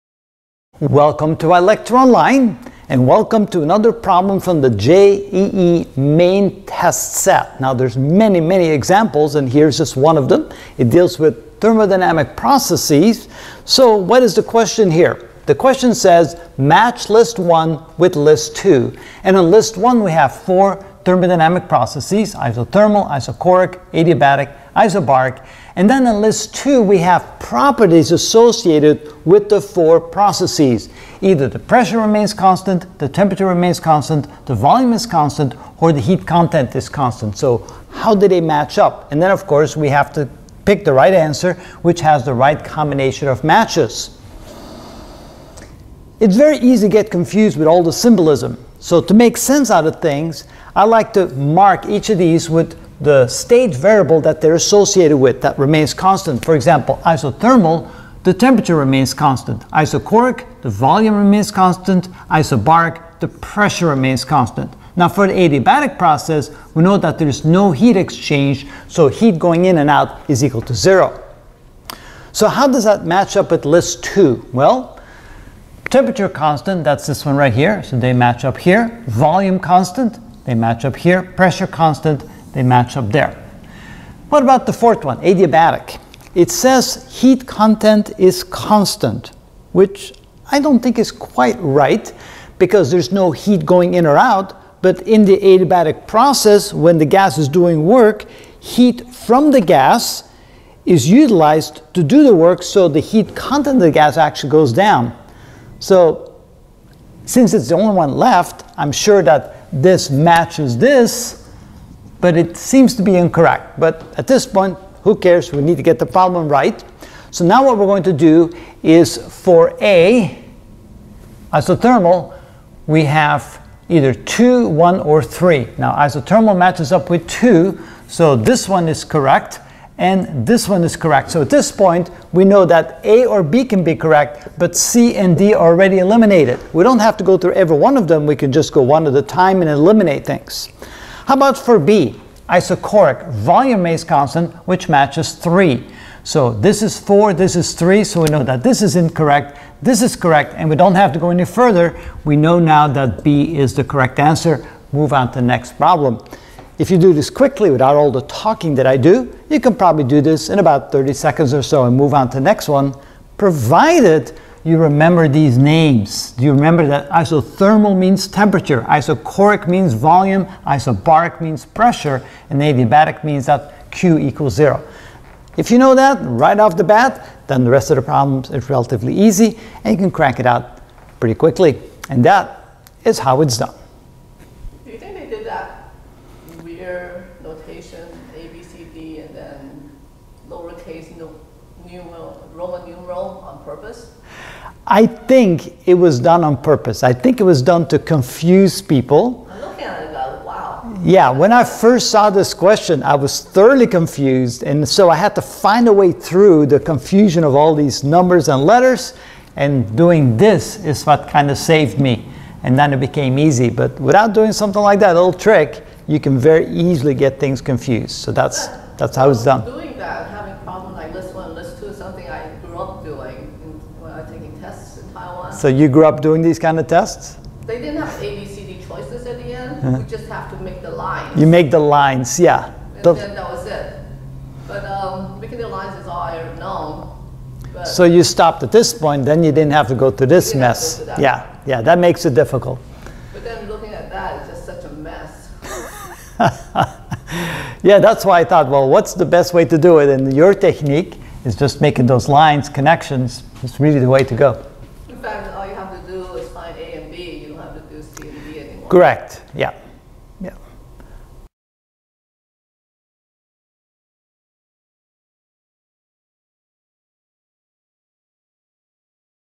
JEE Main Physics Thermodynamics #2 Thermodynamic Process
Match List I with List II:
      List I                            List II
a) isothermal         (i) pressure constant
b) isochoric           (ii) temperature constant
c) adiabatic           (iii) volume constant
d) isobaric             (iv) heat content is constant